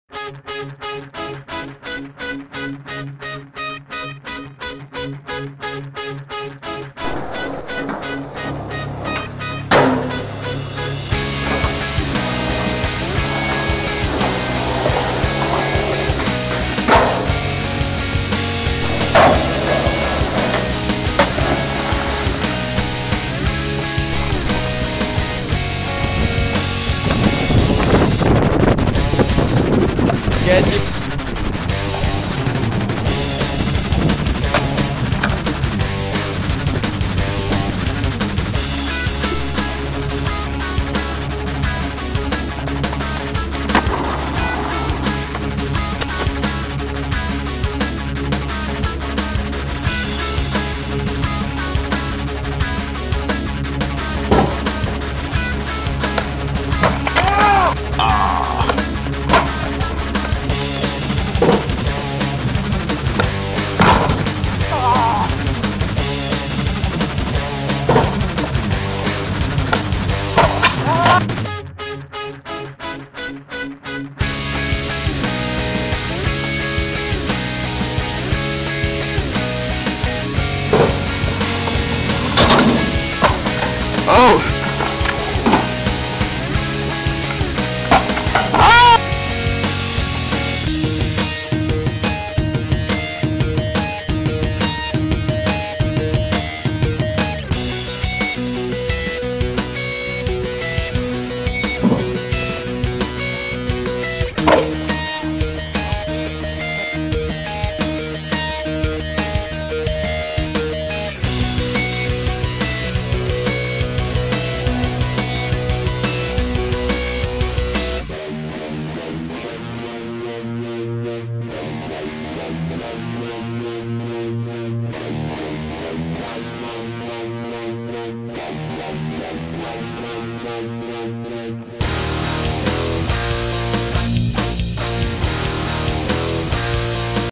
Skate promo!!!! =]
I have been working on this video for some months and haven't uploaded anything due to that. To show I have been working I have some pretty funny bails of some tricks that HOPEFULLY can be in my next part.